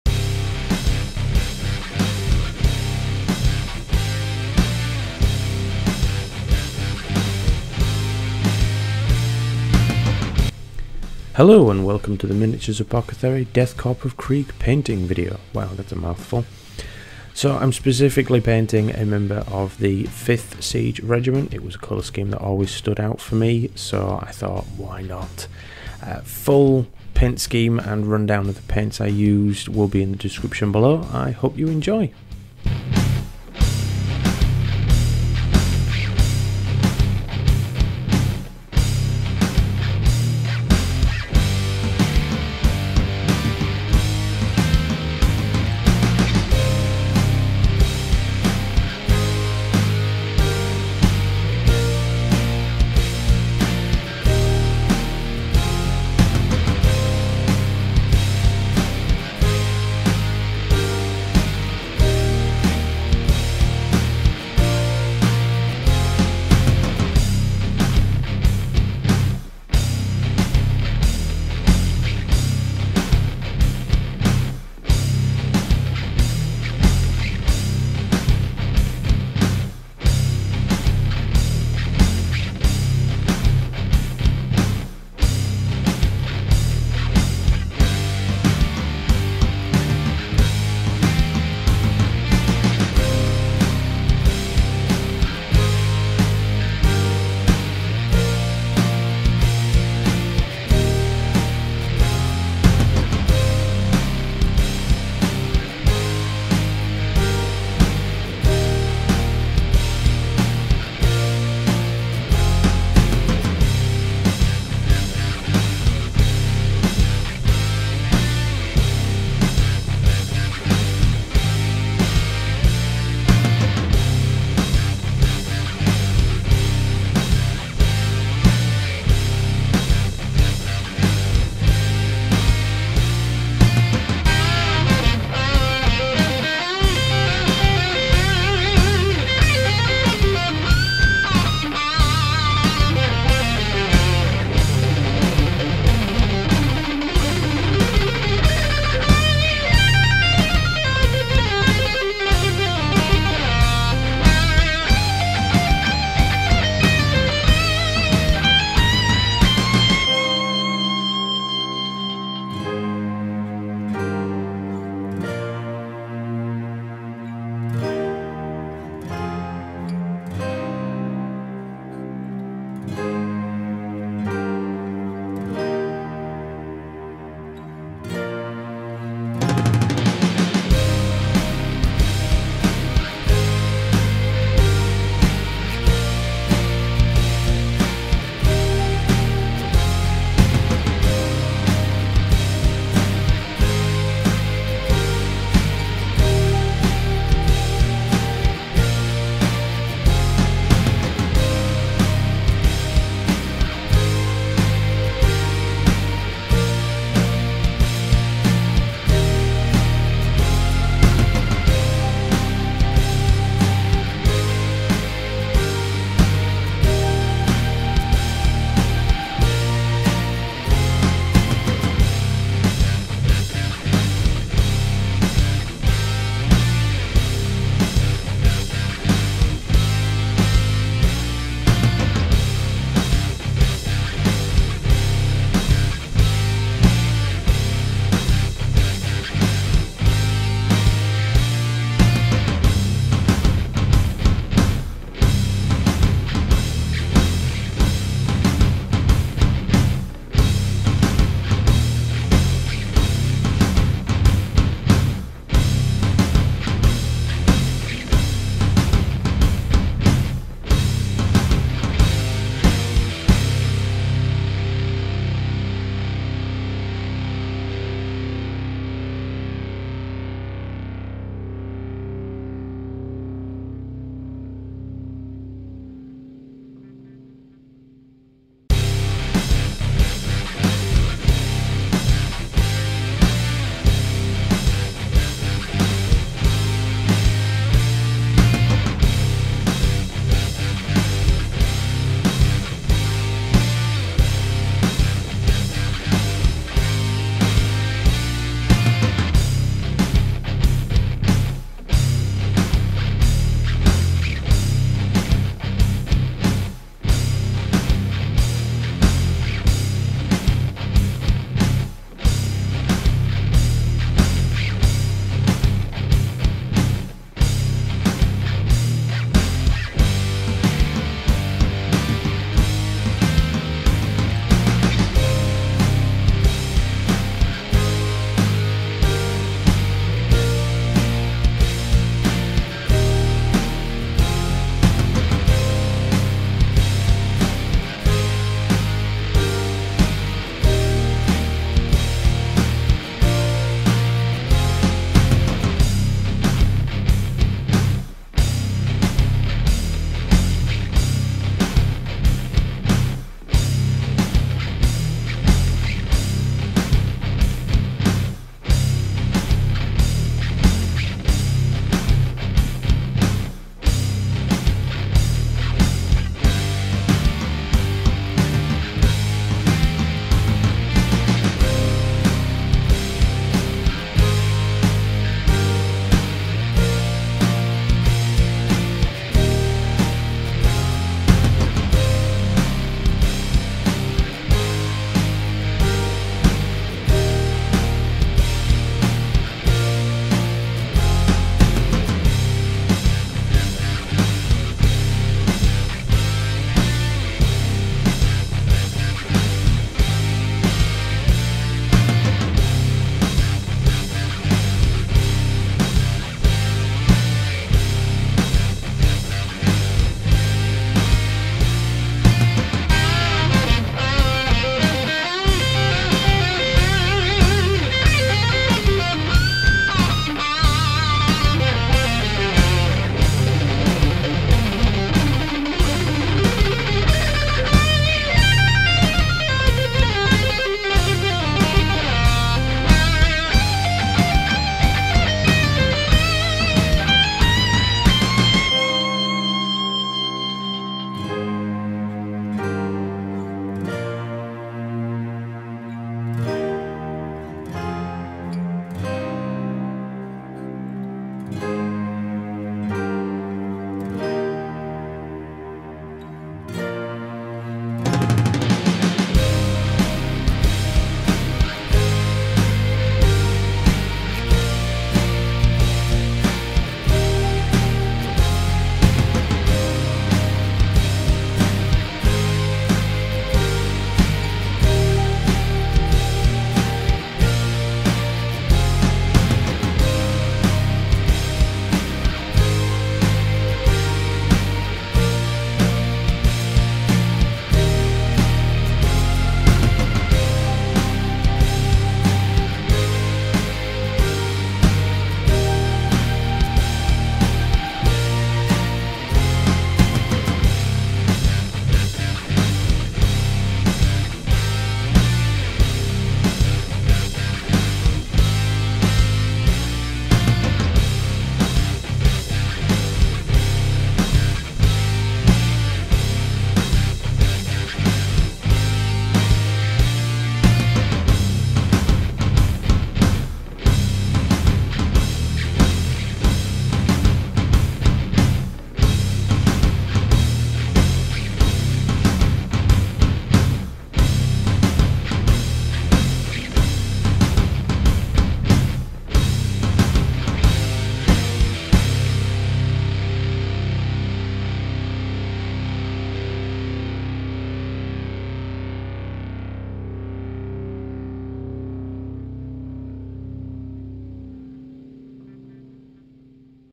The Miniatures Apothecary paints death korps of krieg
Whilst taking some time off from painting the Mastodon interior I thought I'd tackle at least one of the new killteam DKOK for a change of pace.

Really liked the 5th siege regiment colours so thought I'd give it a go.

Colour Scheme:

Prime: Mechanicum Standard Grey
Great Coat: Dark Reaper followed by Nuln Oil wash, highlighted with Thunderhawk Blue.
Gas Mask, Leg Wraps, Bed Role: Zandri Dust followed by Reikland Fleshshade wash and re-highlighted with Zandri Dust.
Leather: Vermin Brown followed by Reikland Fleshshade wash and highlighted with Zandri Dust.
Armour and fatigues: Contrast Black Templar with Thunderhawk Blue highlights.
Sword and Pistol: Leadbelcher followed by Agrax Earthshade Wash and highlighted with Stormhost Silver
Gold: Retributor Armour with a wash of Reikland Fleshshade.
Base: Martian Ironcrust: wash of Agrax Earthshade, dry brush Ryza Rust.

Weathering: 1:2:1 Ryza Rust, Typhus Corrosion, Water for mud spattering.
Dry brush Ryza Rust around feet, legs and lower quarter of great coat.

Battle Damage: Sponge on leadbelcher followed by Typhus Corrosion, lightly edge highlight with Stormhost Silver.